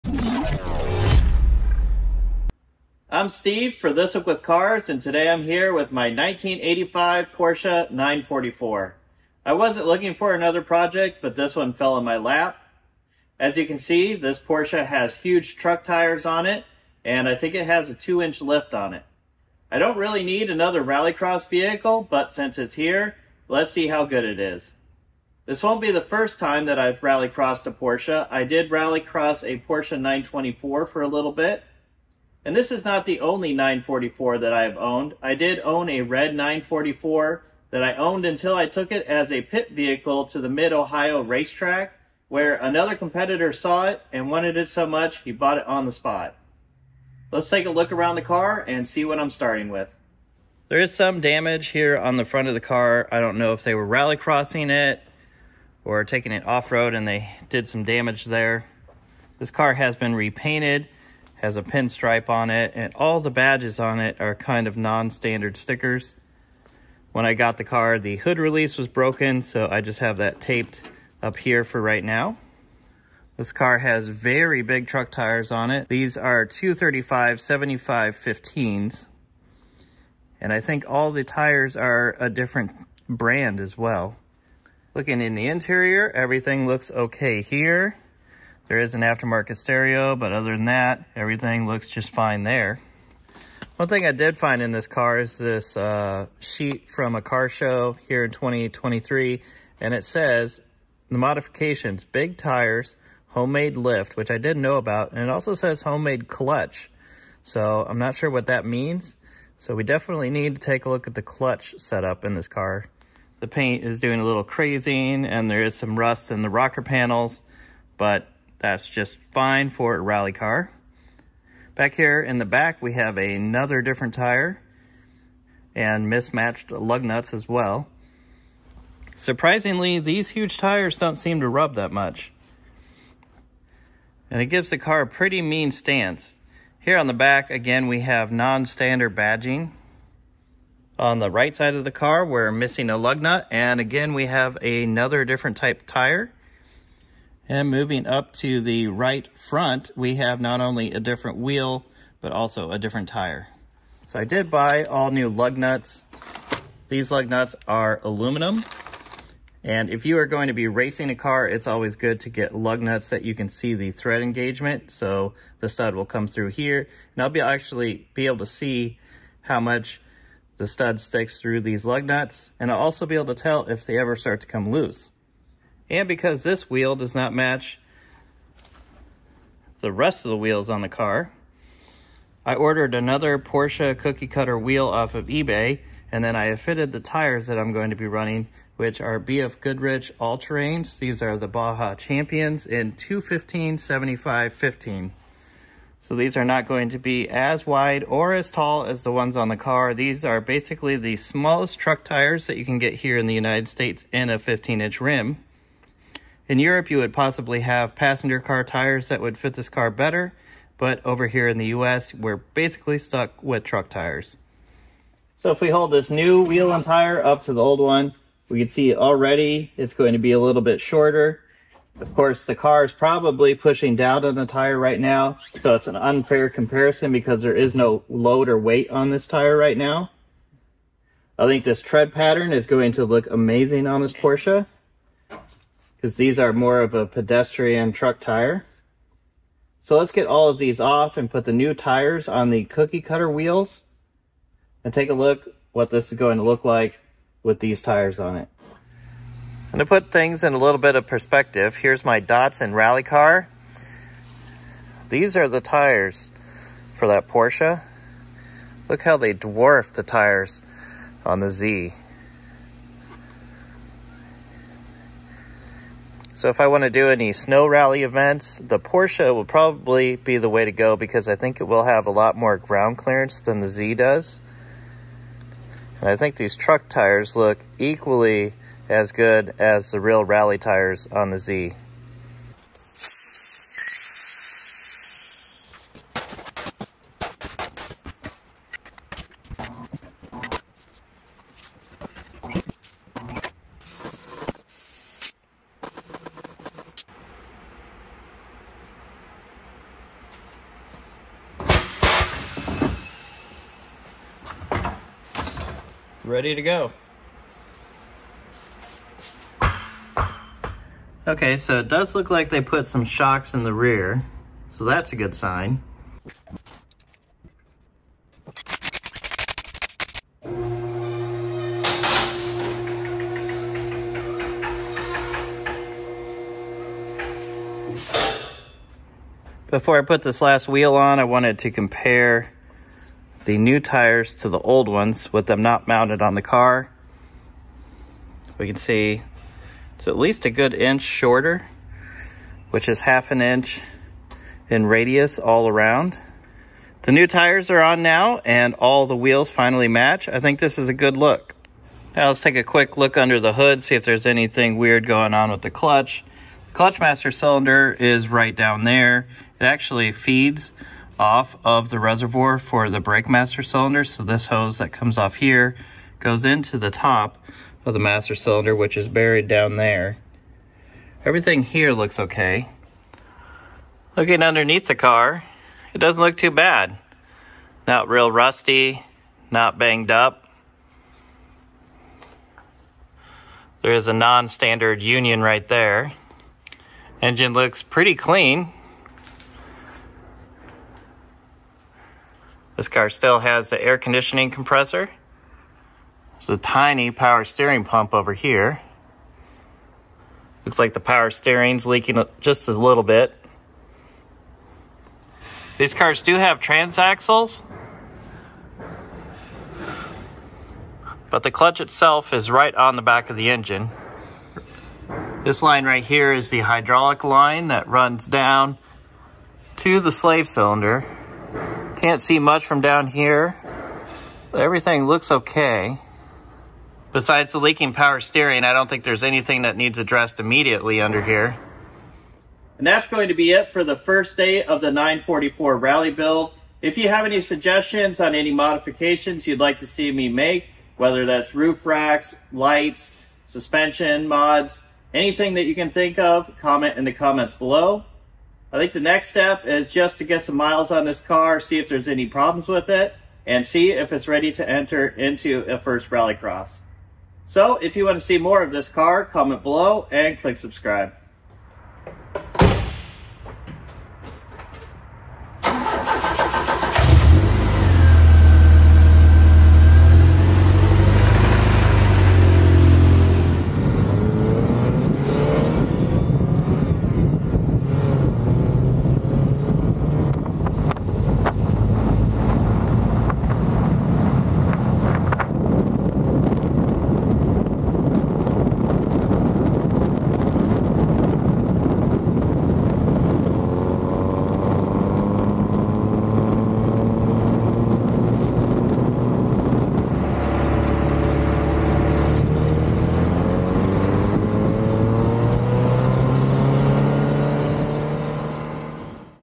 Starting Porsche 944 Rally Car Build
Today I start the Porsche 944 rally car build.  This project was already started by the previous owner but I can improve this.